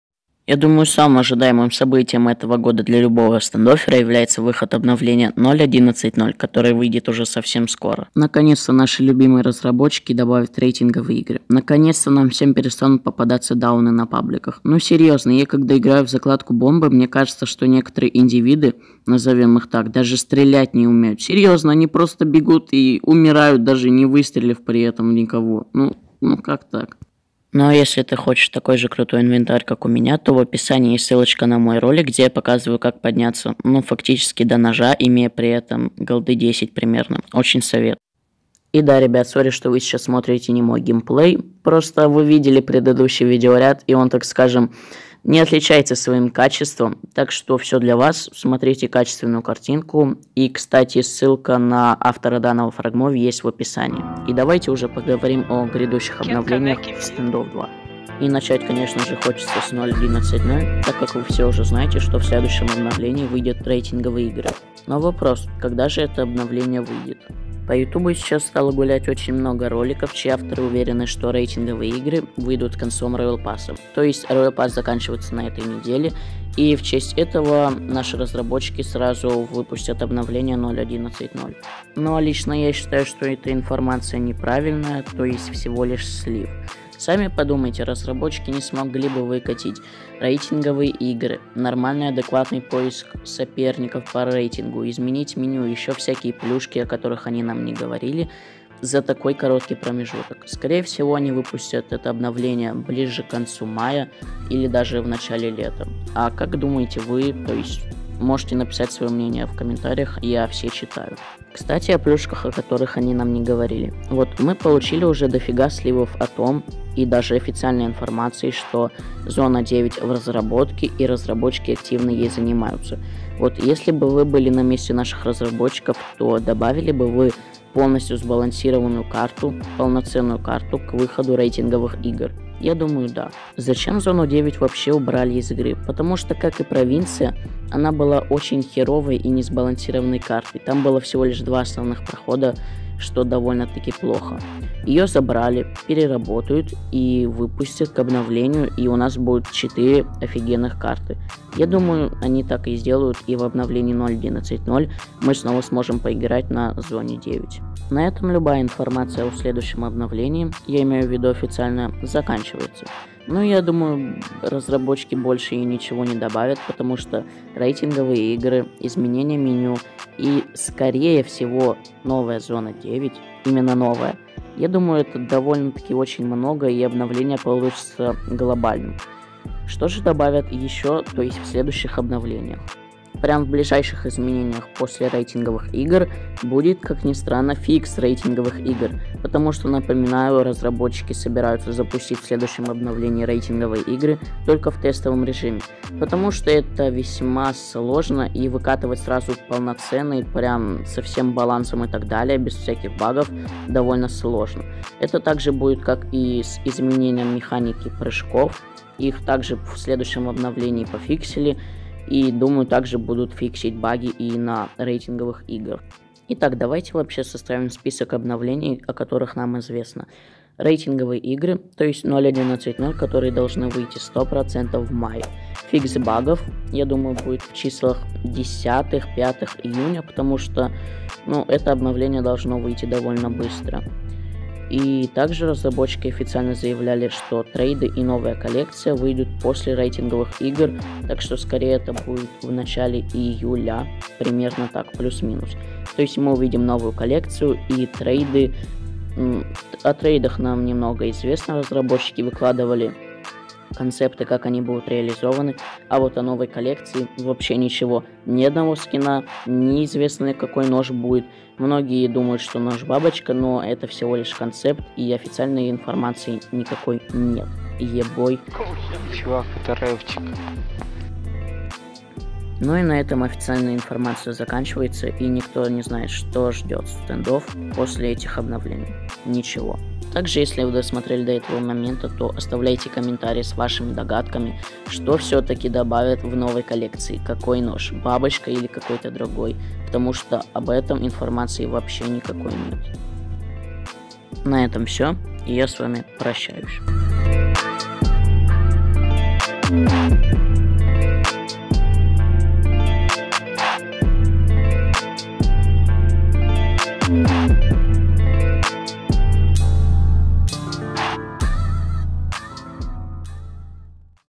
Обновление 0.11.0 в стандофф 2 ! Уже в мае! Рейтинговые игры, трейды, новая коллекция
Обновление в 0.11.0 в стандофф 2 уже в мае! По офицальным заявлениям разрабов будут добавлены рейтинговые игры и будет обновлено меню. Также скорее всего будет добавлена новая зона 9. В следующих обновлениях нас ожидает релиз трейдов и новой коллекции с новым ножом и скином на юсп. Как вы думаете какой нож добавят?)))